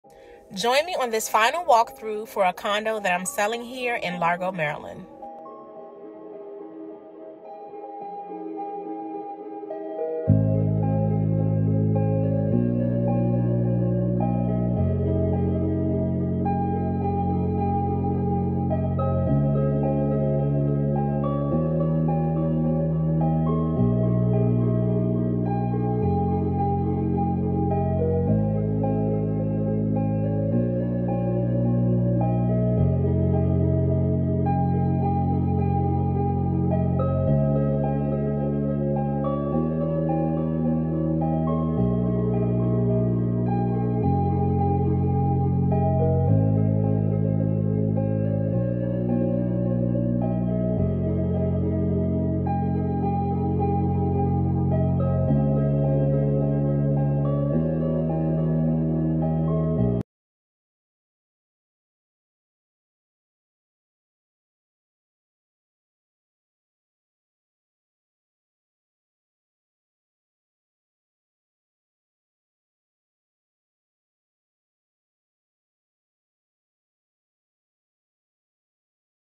Final walk through- Largo, MD
Closing time can be an exciting time, but you should always make time for a final walk-through to make sure that your soon to be new home is in the same condition you expected it would be. Here’s a detailed list of what to check for…

-EVERY room, closet, and crawl space have been checked.

-Requested repairs have been made.

-Any/all warranties are in hand.

-No major, unexpected changes have been made to the property since last viewed.

-All items included in the sale price—draperies, lighting fixtures, etc.—are still on site.

-All appliances and systems are operating (dishwasher, washer/dryer, oven, HVAC, water heater, etc.